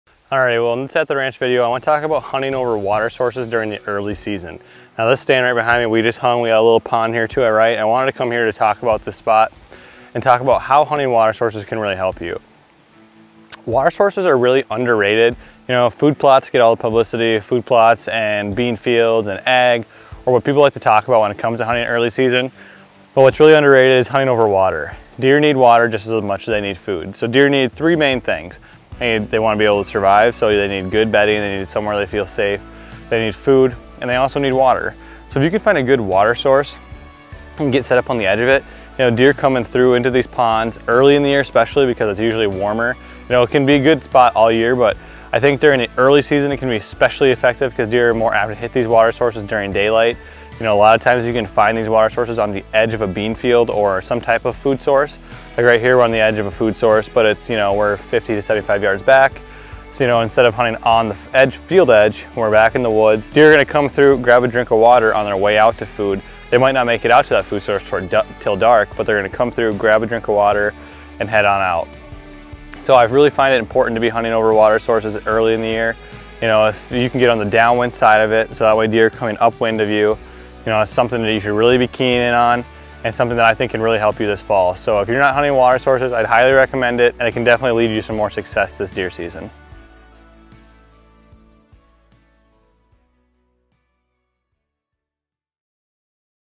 Hunting Water Sources Early in the Fall | At the Ranch | Deer Hunting Tips | Whitetail Hunting
Hunting water sources early in the fall can be a great way to tag a buck. It’s a severely underrated tactic. Deer need water, just as they need food or good bedding cover. If you can find some type of pond or stream just inside the woods along some type of food source, those can be the best. In a lot of cases, deer will swing through and grab a drink of water on their way to the food source. If you’re just hunting the field edge, you may not see the deer as they might not make it to the field until dark. But, if you’re over a water source in the woods, you could be in the game.
Hunting water sources early in the fall is definitely a viable way to shoot a deer.